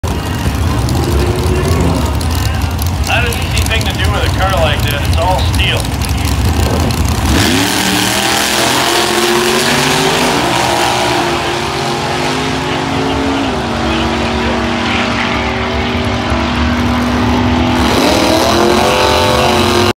(15)TIME😎MACHINE😎NATIONALS | GASSERS | FAMILY STATION WAGON?!?.. 😂🤣
➡️Time Machine Nationals⬅️
Gassers💥Great Lakes Dragaway💥Union Grove, Wisconsin💥Saturday, September 3rd 2022.

➡️Time Machine Nationals⬅️
💥💥9,483 VIEWS💥💥
A lot of Awesome Cars & Races!